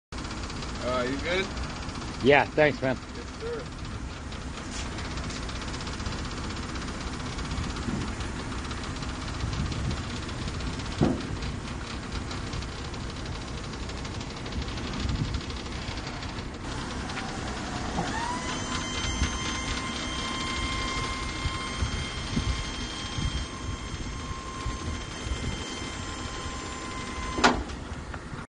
2015 ISUZU NRR For Sale
22' flatbed with stateside and liftgate
This truck is clean, no warning or check engine lights.